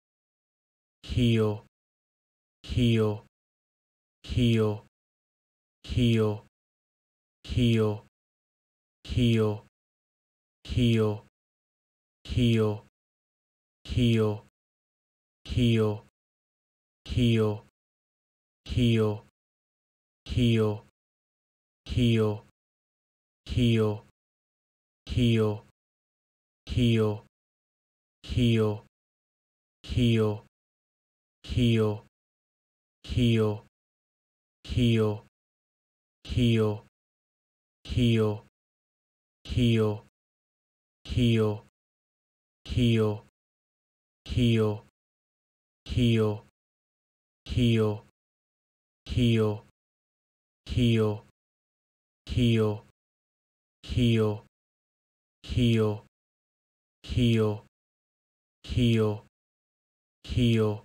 How to pronounce Heal||How to say Heal||Heal Pronunciation||ABDictionary
Do you want to know how to pronounce Heal? In this video you'll learn how to say or pronounce Heal for free